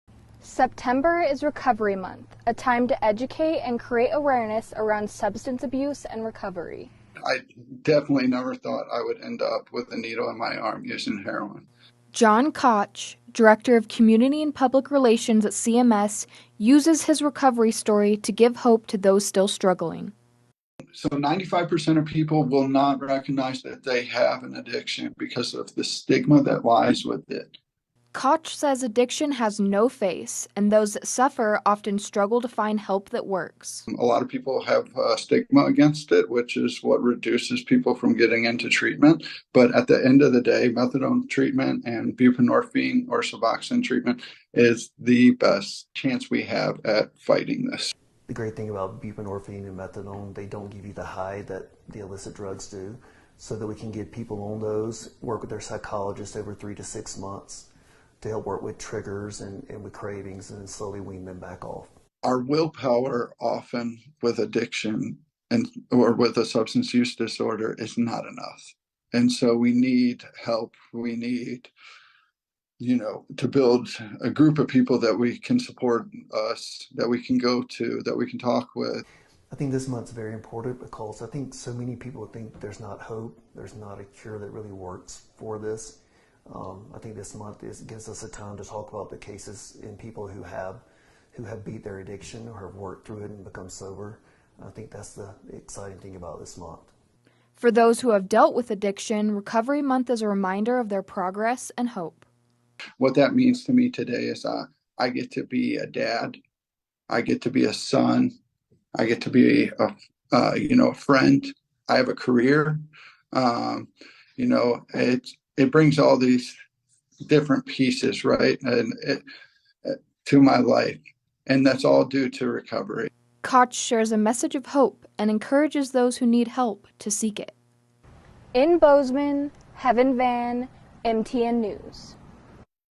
A message of hope for Recovery Month from those recovering from addiction
September is recovery month—a time to educate and create awareness around substance abuse and recovery.